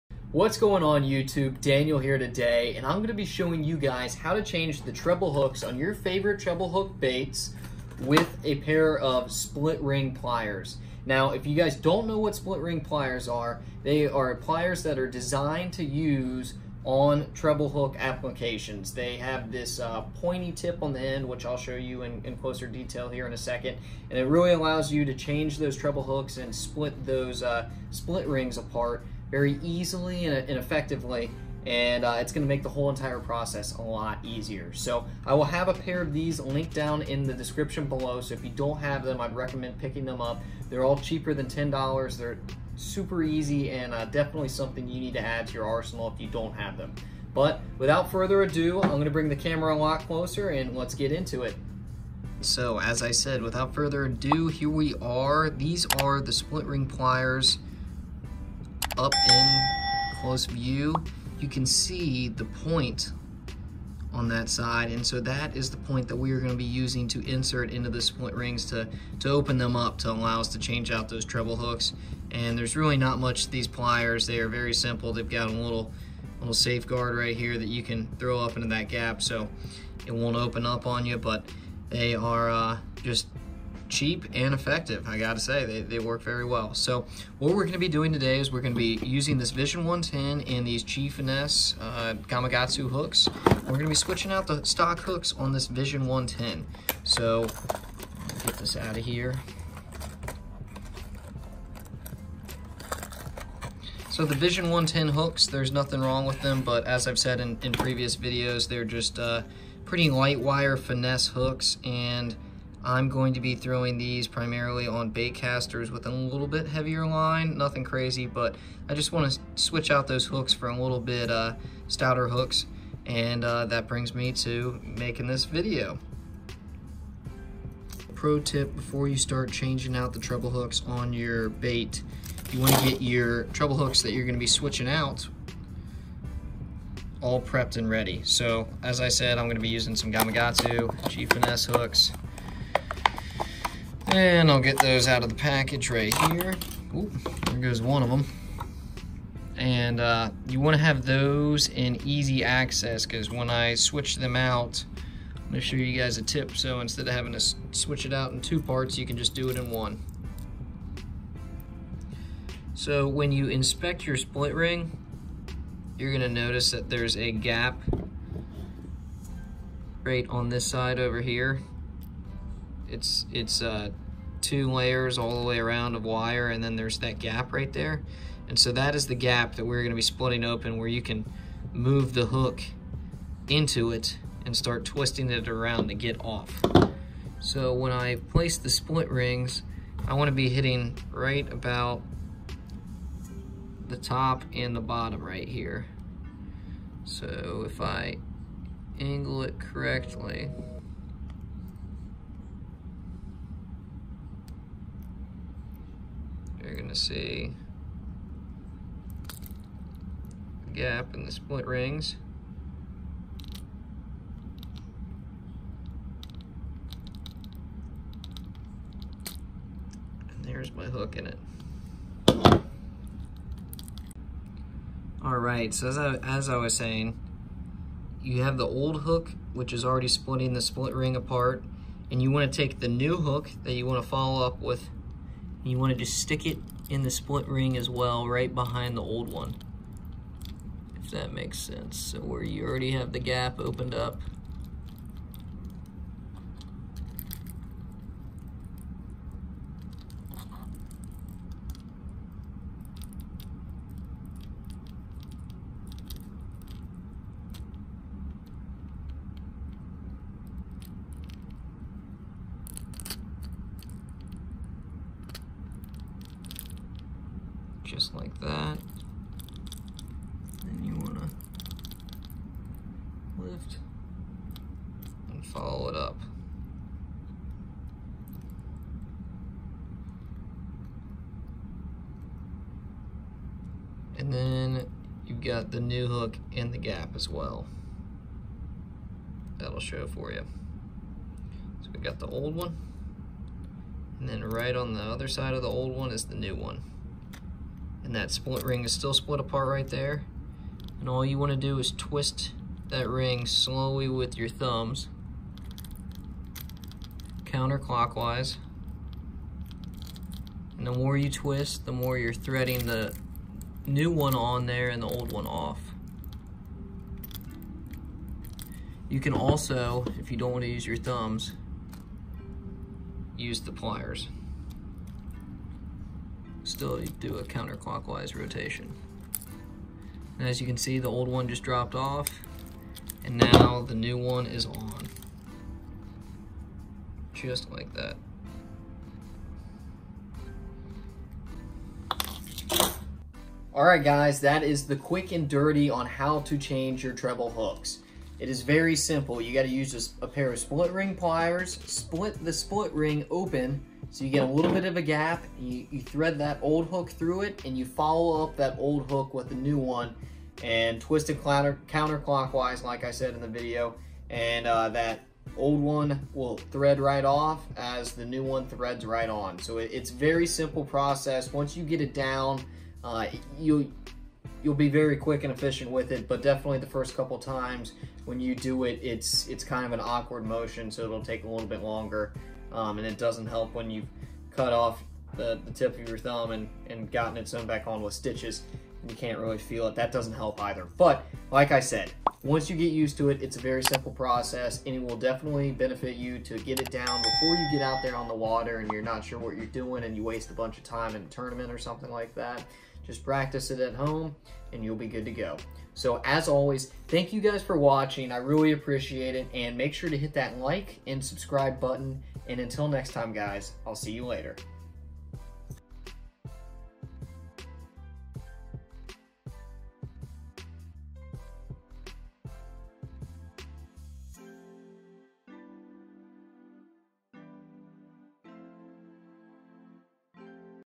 How to Change Treble Hooks with Split Ring Pliers
Hey guys!

In this video I show you how to swap out the old treble hooks on your bait with new treble hooks. In order to do this effectively, I recommend you use a pair of split ring pliers. (they were designed for this application). After you do this a few times, you'll realize it doesn't take long at all to swap out the treble hooks on your bait. I've linked a pair of pliers further down in the description as well.

Split Ring Pliers:

Megabass Vision 110:

Treble Hooks: